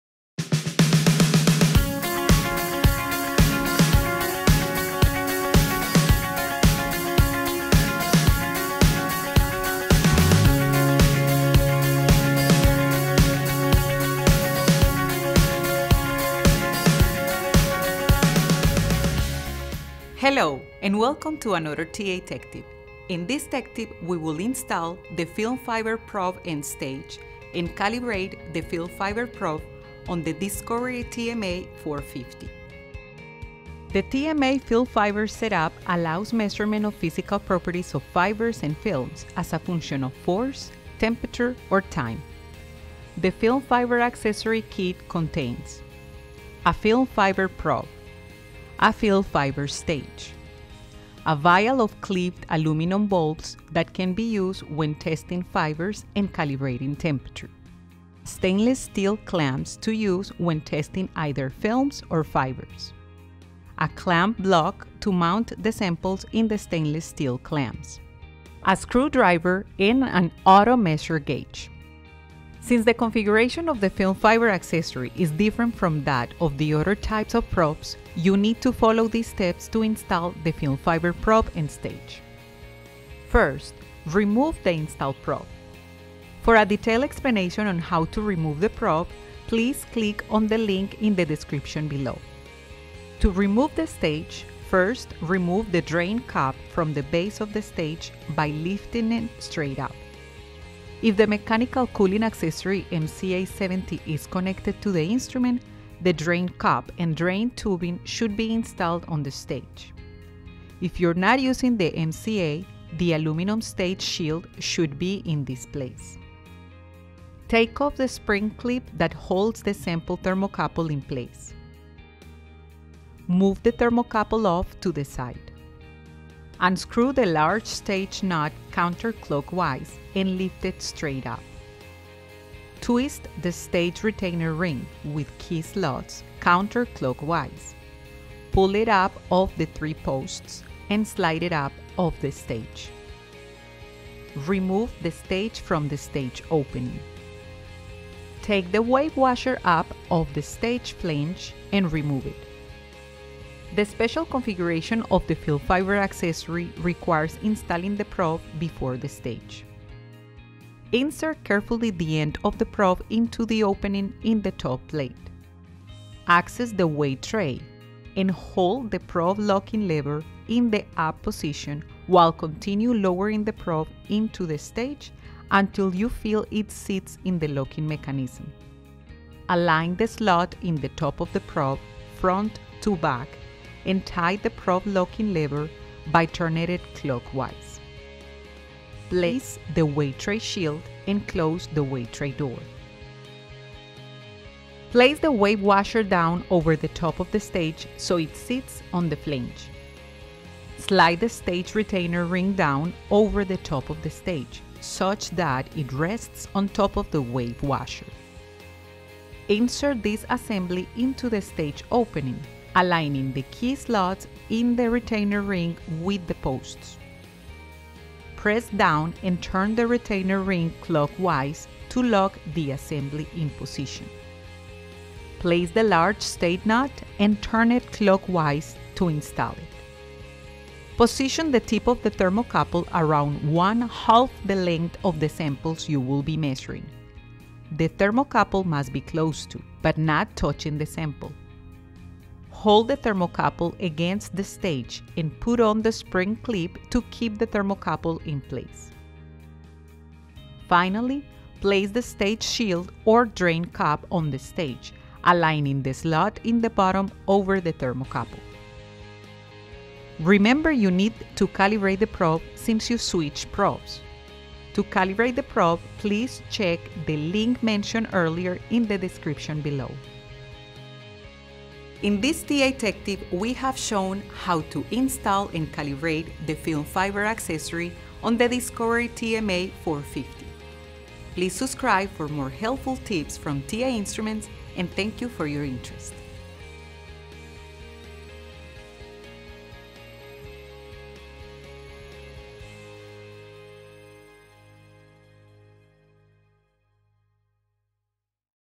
Installation of the Film/Fiber Probe on the Discovery TMA 450
In this TA Tech Tip, we show you how to install the film/fiber probe for the Discovery TMA 450 thermomechanical analyzer.